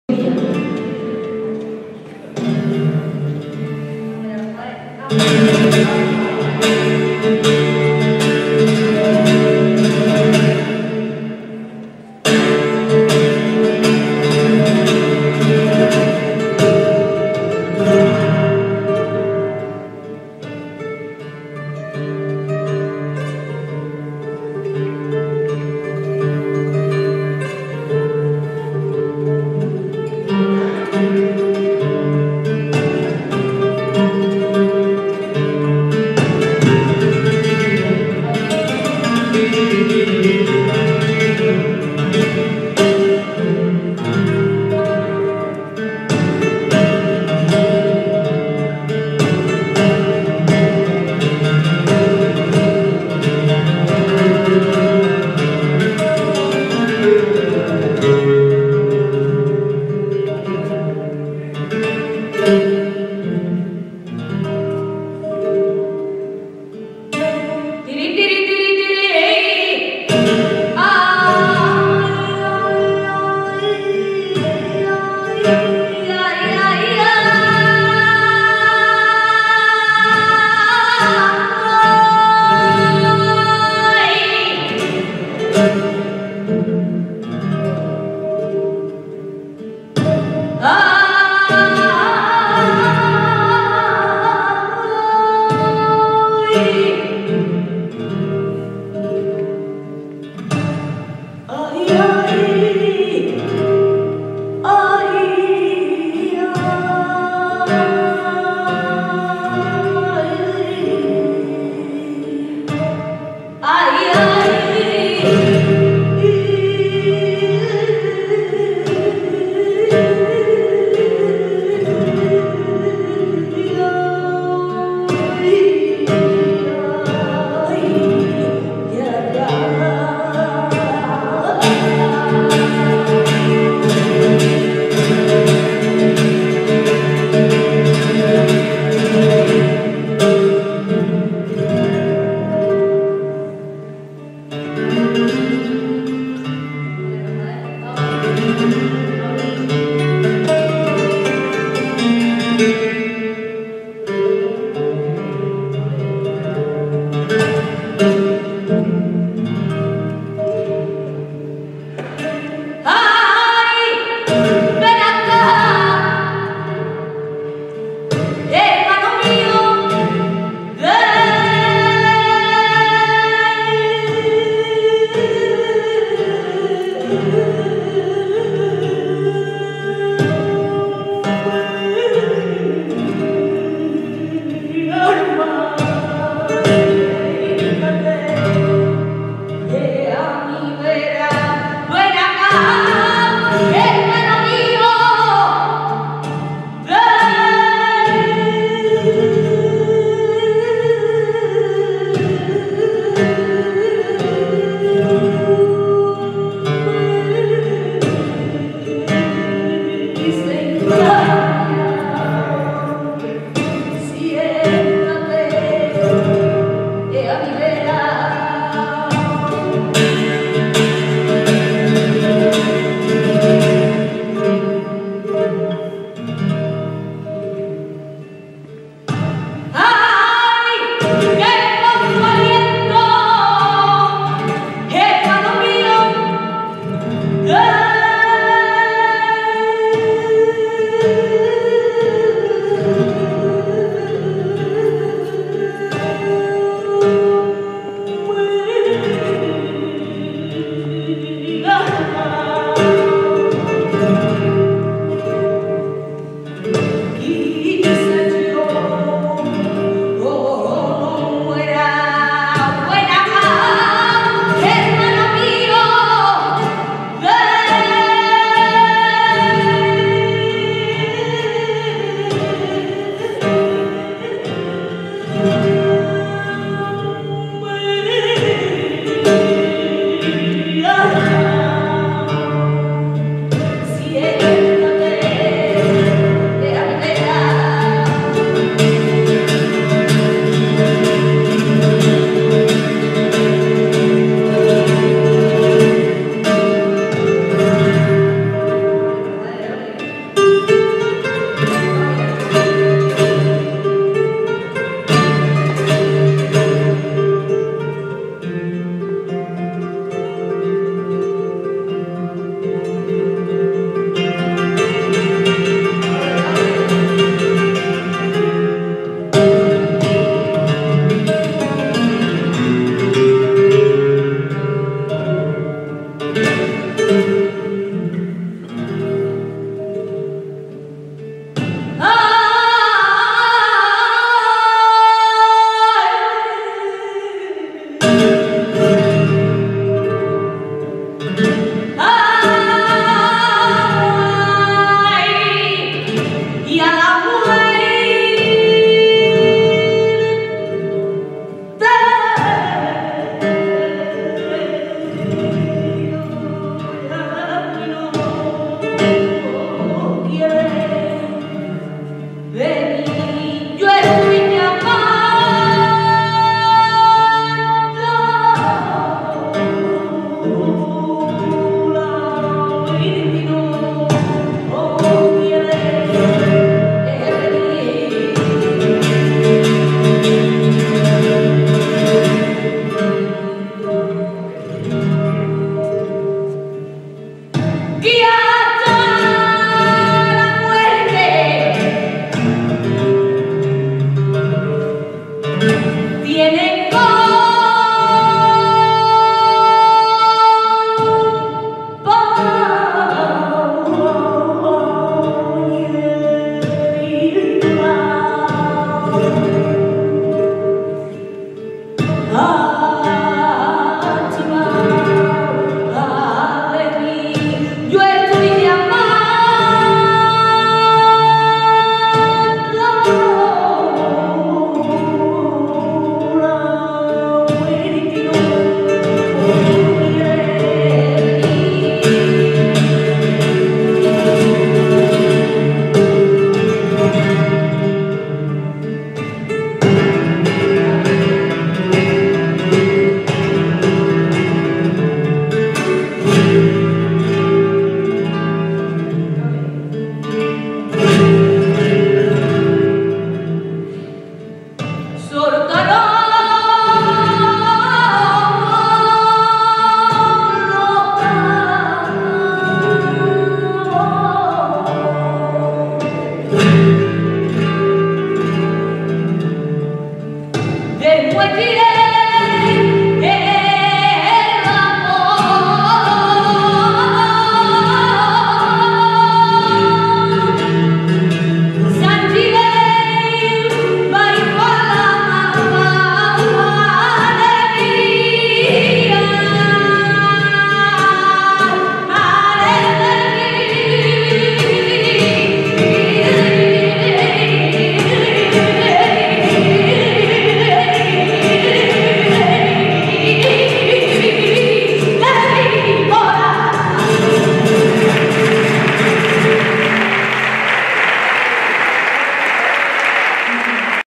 FLAMENCO.-AL CANTE :CARMEN CARMONA Y AL TOQUE RAFAEL TRENAS
EL P. VYB.- ORG.- FEDERACIÓN DE PEÑAS DE CÓRDOBA Y EL AULA FLAMENCA DEL REAL CIRCULO DE LA AMISTAD DE CÓRDOBA.